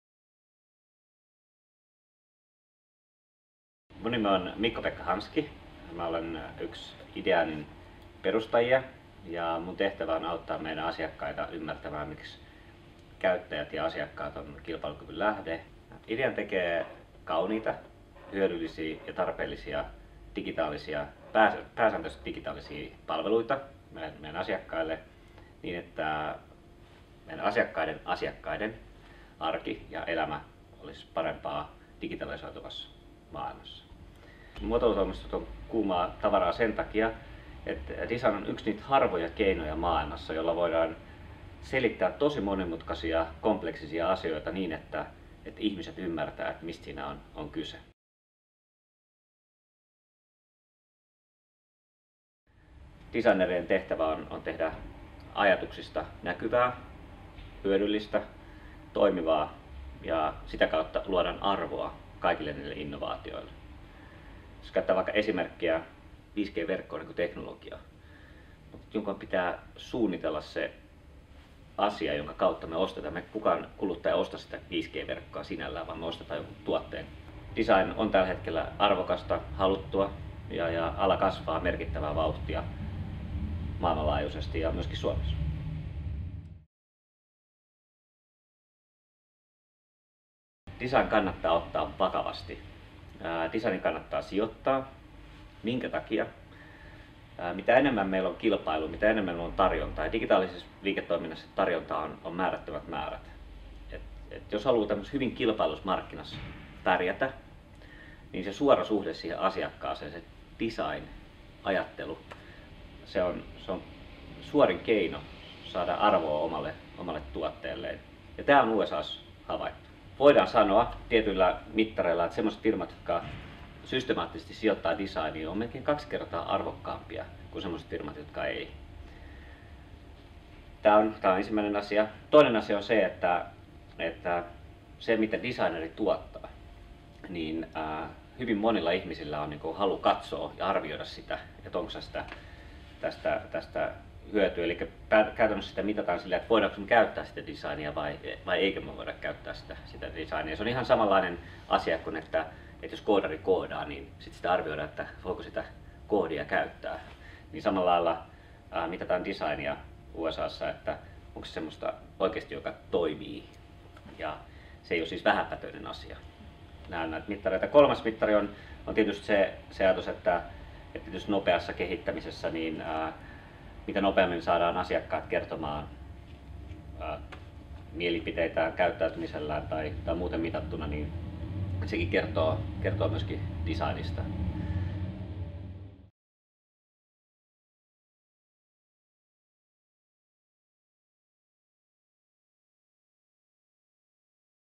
Mikko-Pekka Hanski, yksi SuomiAreenan Suomi suosta! -keskustelun puhujista
Ideanin Mikko-Pekka Hanski puhuu muotoiluosaamisesta. Mikko-Pekka Hanski mukana myös Design Forum Finlandin ja Myynnin ja markkinoinnin ammattilaiset MMA:n keskustelutilaisuudessa SuomiAreenan Purje-lavalla pe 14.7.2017 klo 11.45–13.00